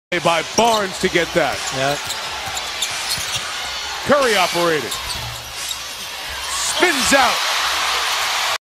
NBA, playoff 2015, Warriors vs. Grizzlies, Round 2, Game 6, Move 35, Stephen Curry, funny
Golden State Warriors - Memphis Grizzlies, Game 6, 15/05/2015, quarter 3
Stephen Curry, funny